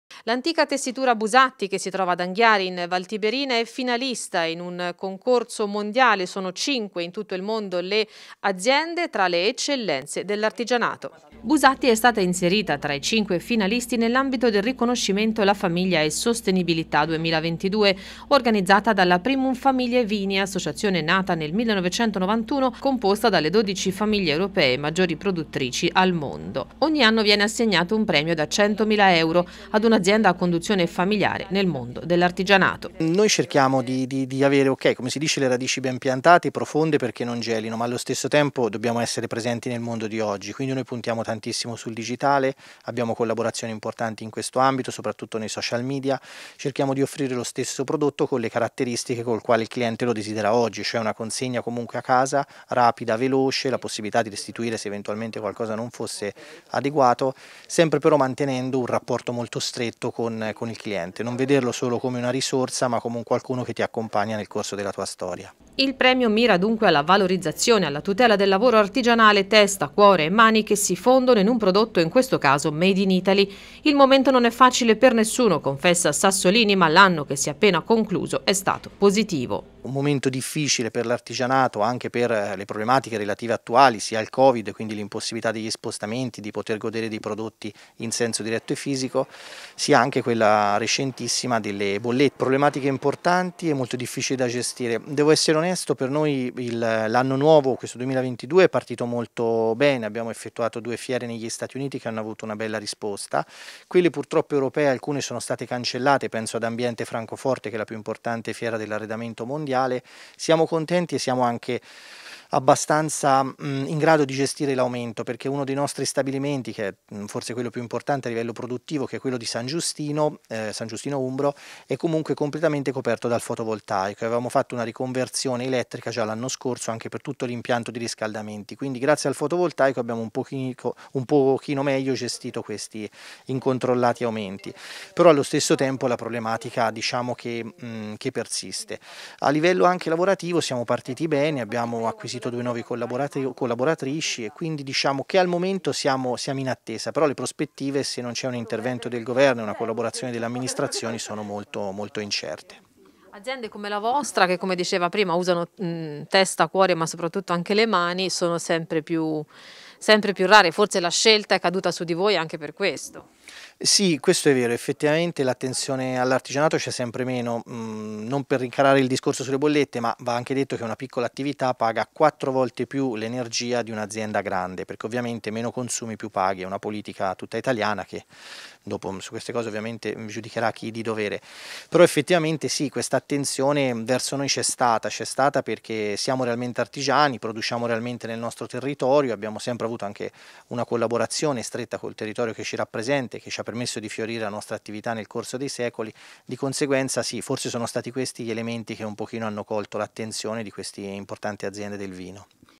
Busatti finalista nel concorso "La Famiglia è Sostenibilità"
Busatti è inserita tra i cinque finalisti dell’ambito riconoscimento "La Famiglia è Sostenibilità" 2022, organizzata dalla Primum Familiae Vini, associazione nata nel 1991 e composta da dodici famiglie europee produttrici nelle maggiori regioni vinicole del continente. Ogni anno viene assegnato un premio di 100mila euro a un’azienda a conduzione familiare di qualsiasi settore che si sia distinta per una serie di iniziative lodevoli in ambito di sostenibilità, innovazione, artigianato e continuità generazionale.

Il premio mira dunque alla valorizzazione e alla tutela del lavoro artigianale, testa cuore e mani che si fondono in un prodotto in questo caso made in italy. Il momento non è facile per nessuno, confessa Sassolini, ma l'anno che si è appena concluso è stato positivo

"Crediamo fermamente che le aziende familiari siano una colonna portante delle economie regionali e nazionali" si legge nei canali web ufficiali di Fpv. "Una grande soddisfazione essere finalisti di un’iniziativa che promuove su scala internazionale le peculiarità che caratterizzano le attività a conduzione familiare – ha commentato Livio Sassolini, ad di Busatti – Il vincitore del premio sarà comunicato a marzo, ma già trovarsi tra le cinque attività selezionate è per noi una bella vittoria. Un ringraziamento agli organizzatori e a tutti coloro che in questi anni hanno permesso a Busatti di promuovere una filosofia aziendale votata alla tradizione e con uno sguardo verso il futuro".